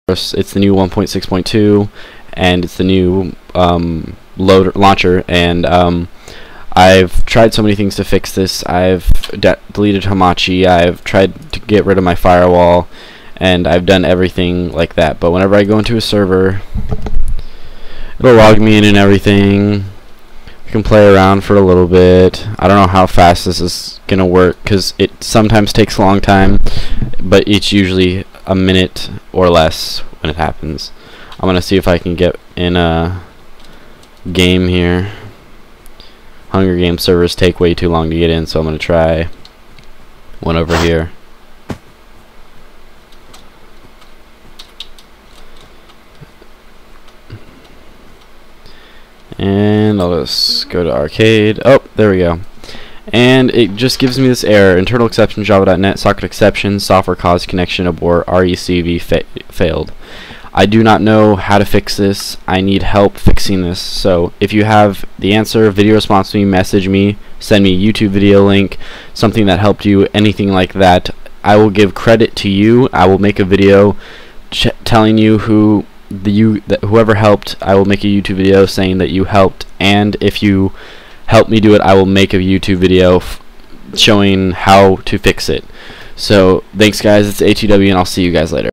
internal exception:java.net.scoketexception:Software caused connection abort:recv failed
Thanks for watching or at least clicking on my video it helps alot if you have an answer to my problem please leave a video response or a message or even a comment anything helps